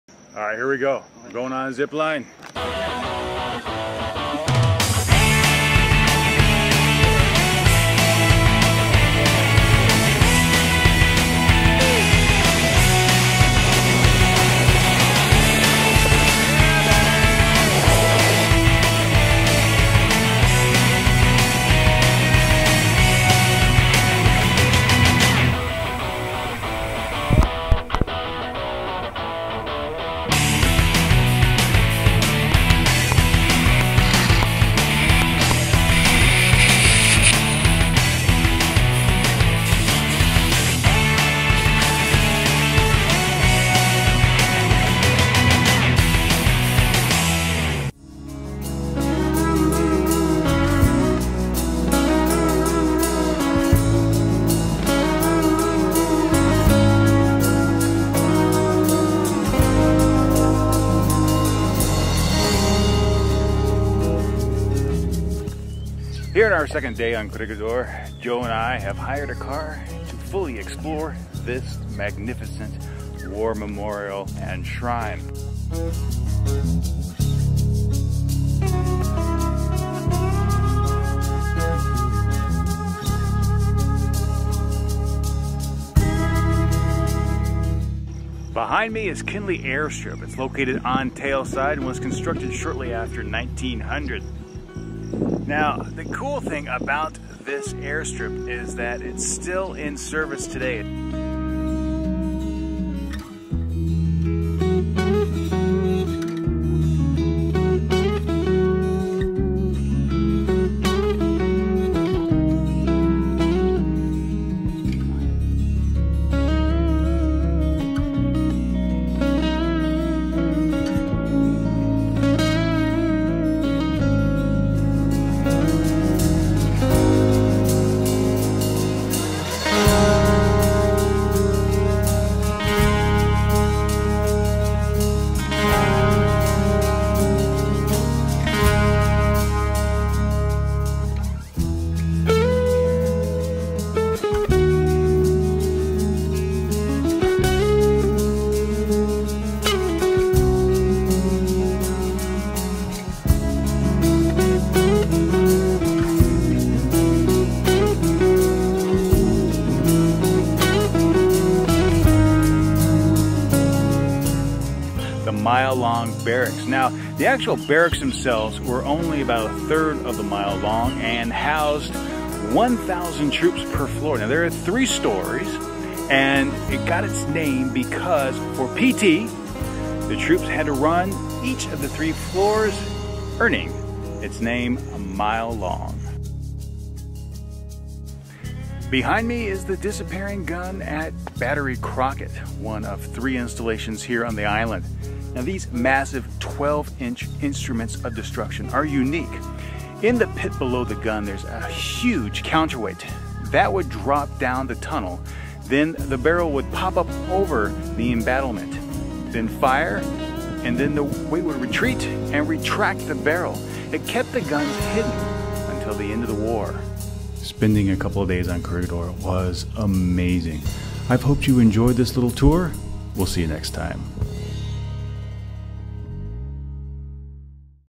Corregidor: Day Two
Our second day on the island was by far the best. We hired a car and drive for a more personalized tour, and ever-so-thankful that we did.

Music, fully licensed.